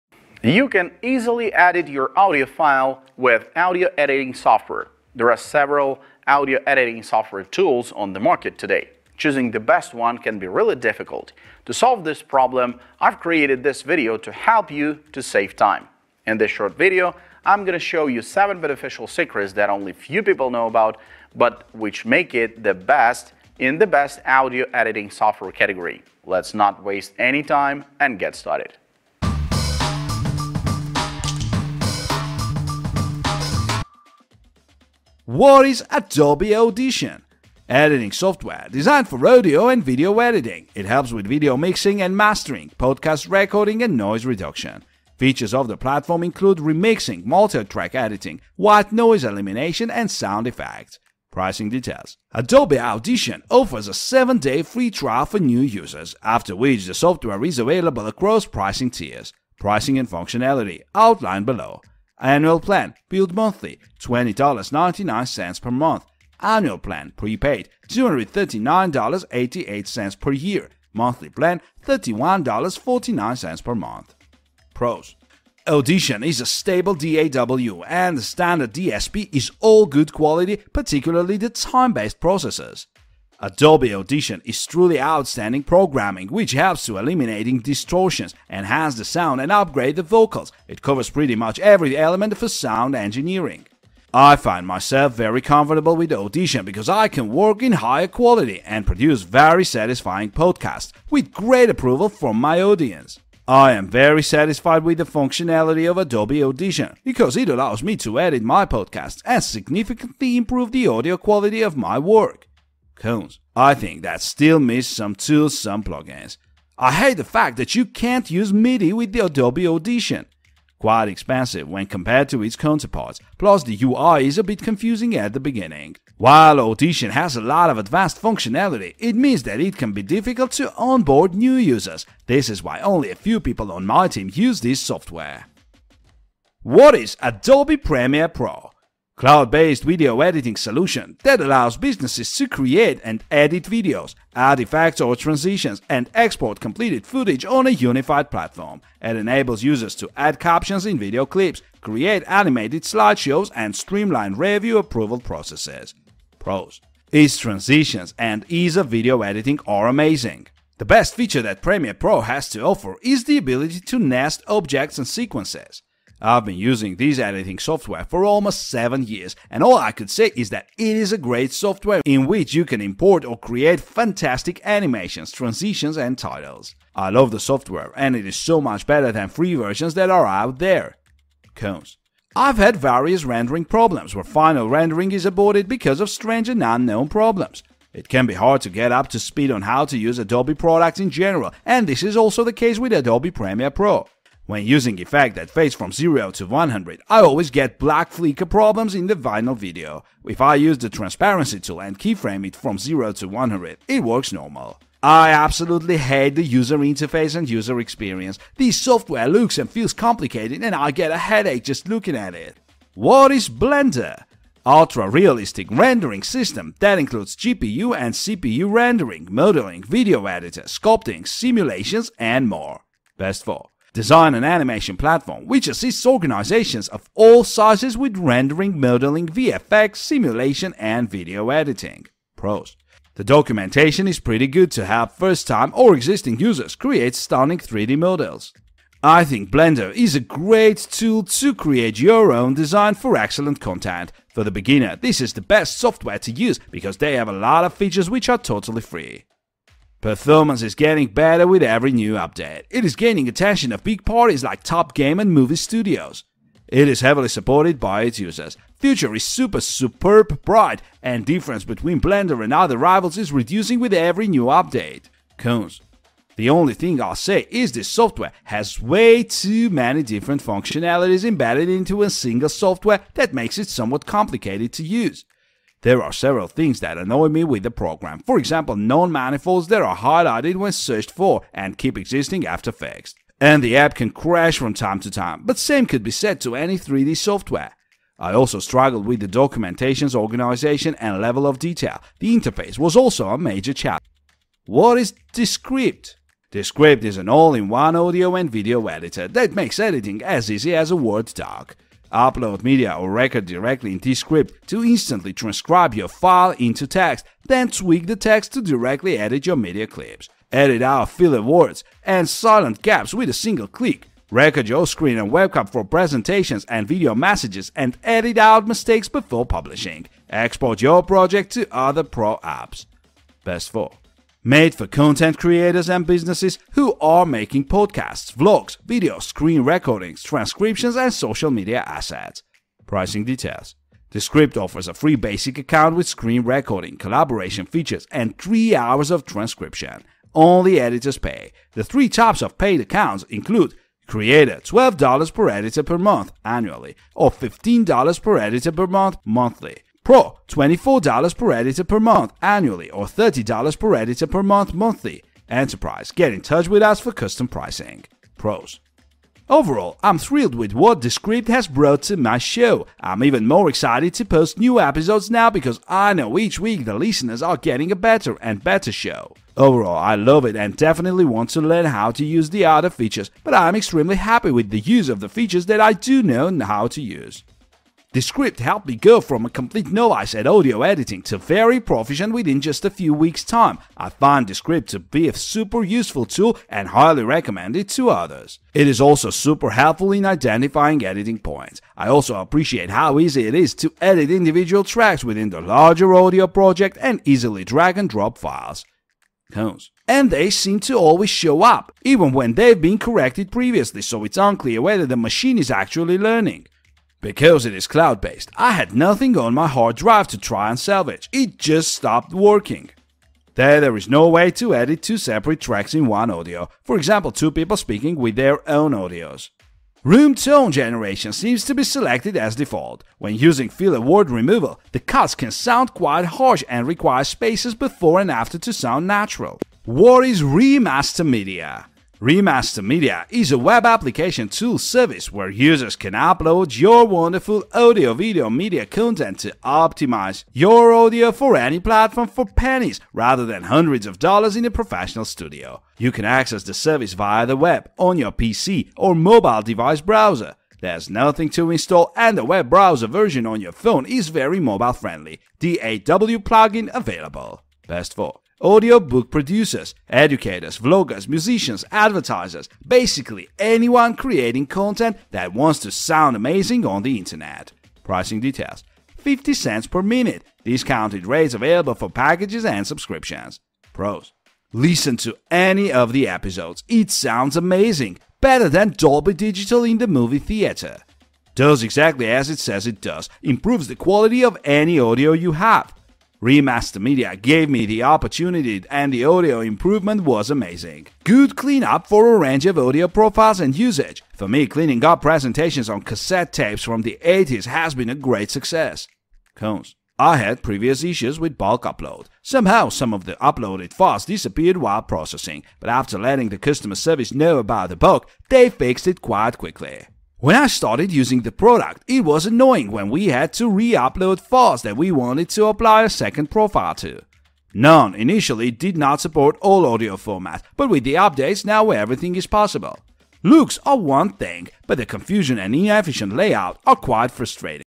Top 5 Audio Editing Software - How to Edit Audio Like a PRO
In this video I have shared with you top 5 best audio editing software for laptop and pc . For audio editing you need of an audio editor. In this video I will give you 5 best audio editor for pc and you can start to edit audio and making music like a pro. In this video we will get acquainted with such audio editors as Adobe Audition, Adobe Premiere Pro, Blender, Descript, ReMasterMedia